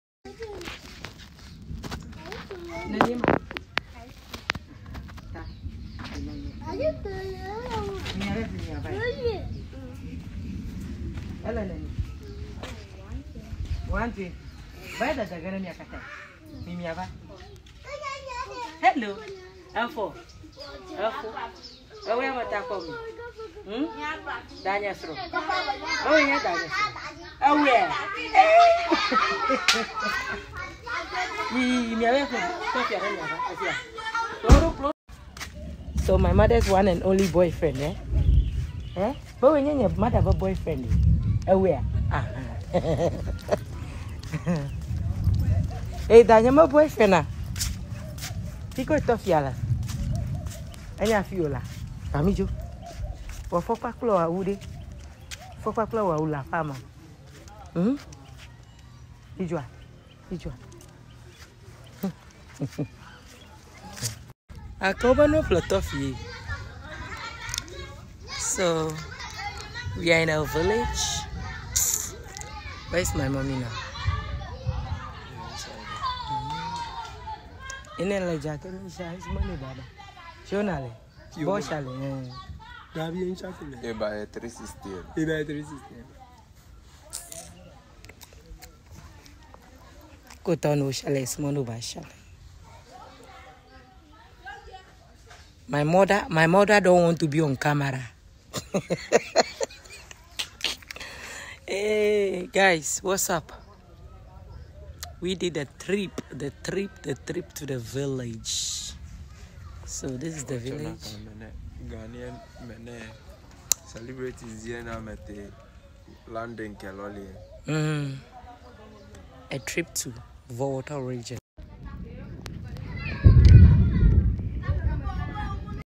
OUR LIFE WITH THE LITTLE ANGELS IN OUR MOMS VILLAGE IN THE VOLTA REGION 🇬🇭🇬🇭
Hello guys welcome to my channel today we are going back to Accra I have to do little things on my house please don’t forget to subscribe to the channel for more videos videos thanks 🙏